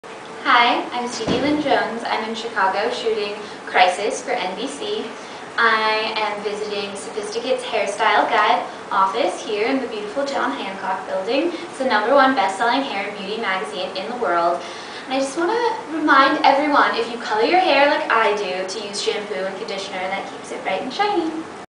STEVIE LYNN JONES VISITS WITH SOPHISTICATE'S!
Actress Stevie Lynn Jones, star of the upcoming NBC series Crisis, stopped by to visit with Sophisticate's Hairstyle Guide today in Chicago. We can't wait to see this gorgeous star in primetime, but until then, she's sharing a great hair tip with us...